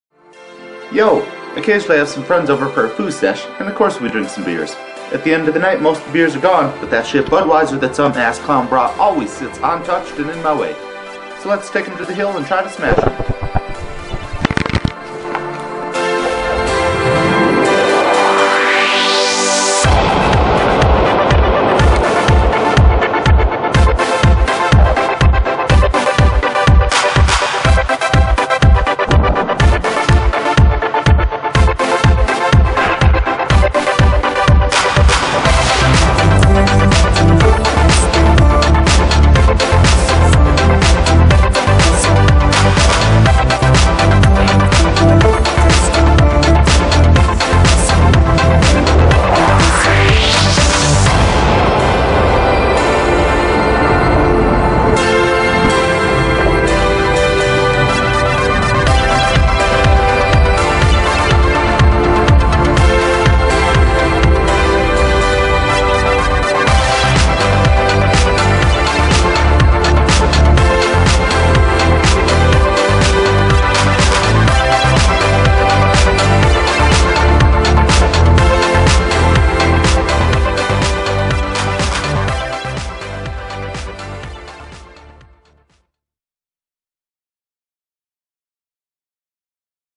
Smashing "Buds"
Consumer Reports: Coors tastes better than Budweiser
Taste off: Coors had a clean, good balanced taste with citrus notes, while Budweiser had slight off notes (tin and sulfur) and lighter, yeasty notes. Budweiser was also a bit astringent tasting.
+ Coors has sponsored a bunch of the foosball tournaments I've ran.  :)
Please "like" if ya like, and watch in HD!